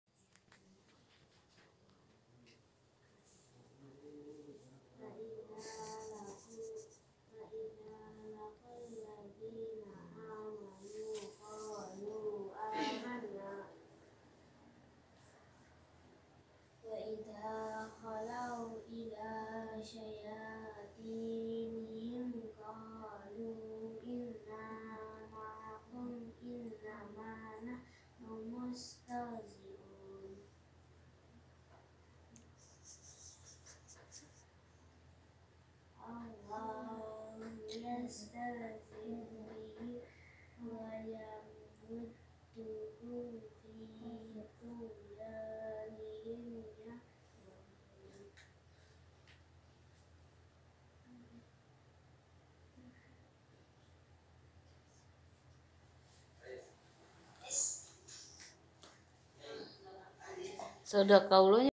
Tasya ajarin tiara mengaji
Tasya ajarin tiara mengaji dirumah